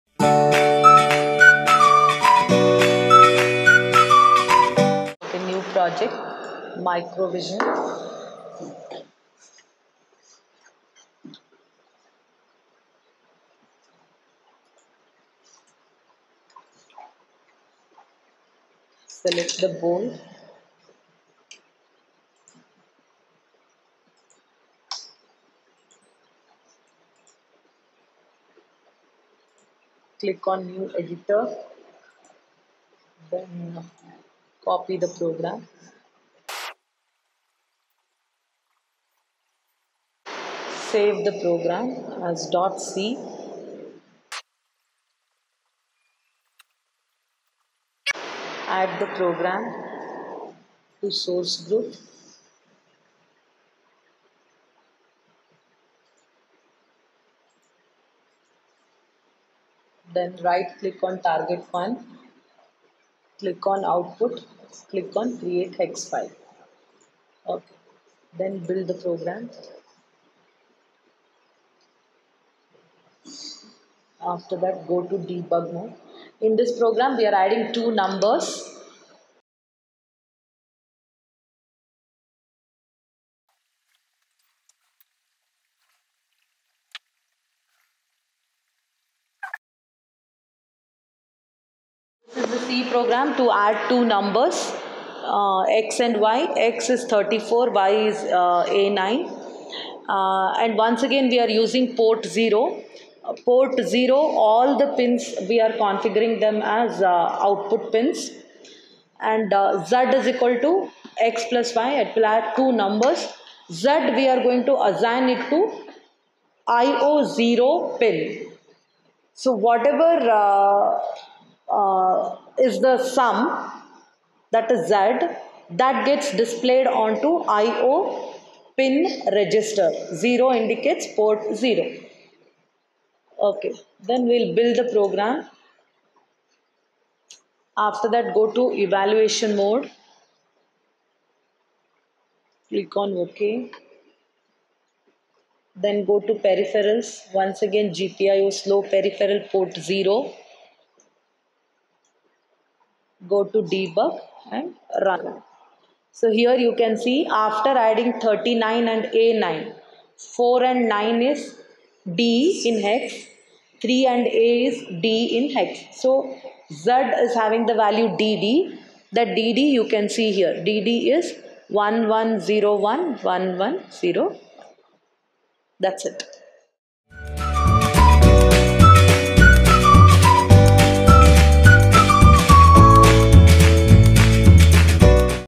Addition of Numbers - C Program Simulation for ARM | Prof. Keerti Torvi, Pesit-BSC
Procedure explained by Prof. Keerti Torvi, Assist. Prof, Pesit-BSC.

This is 7th Program of Microprocessor & Micro controller Lab (Vtu):

Simulation of C Programs for ARM microprocessor using KEIL -  Addition of Numbers.